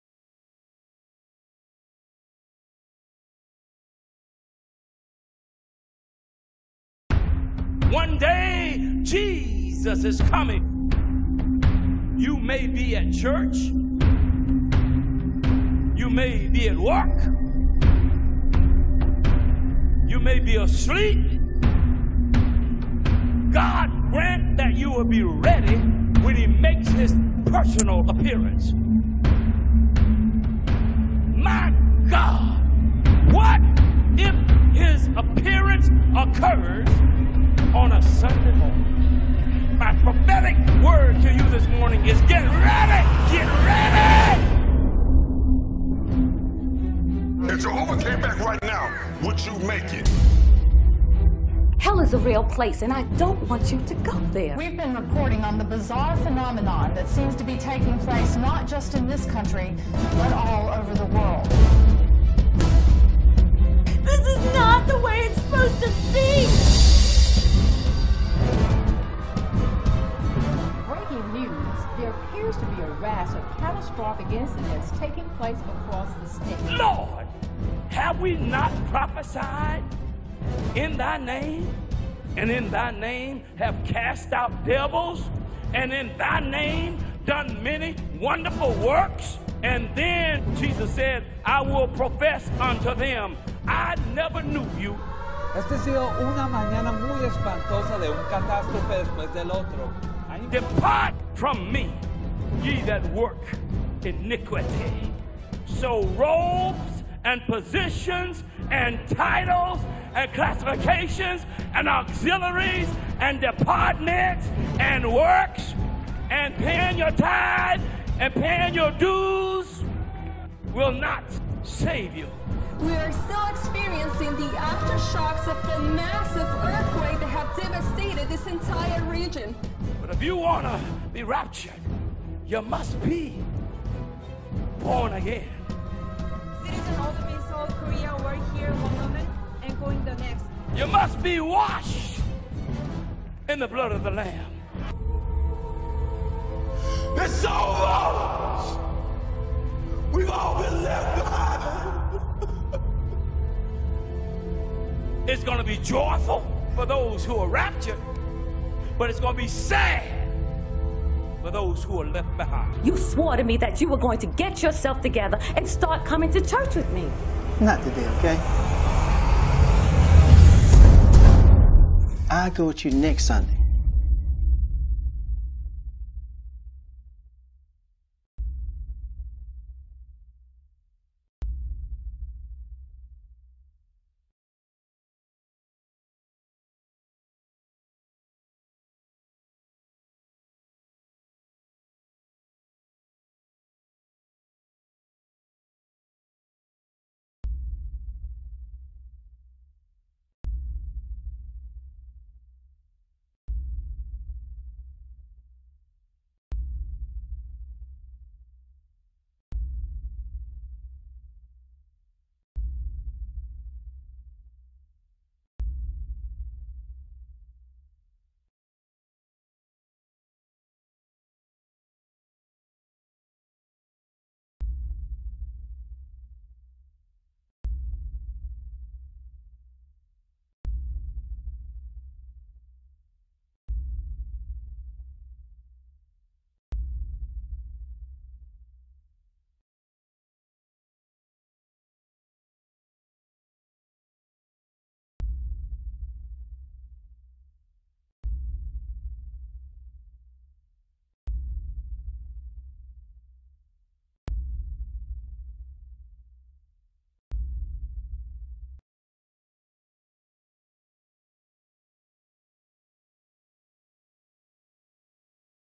Entrückung verpasst?!? Teil 1 Left behind?
Was ist die Entrückung? Was sind die Anzeichen? Was beginnt nach der Entrückung?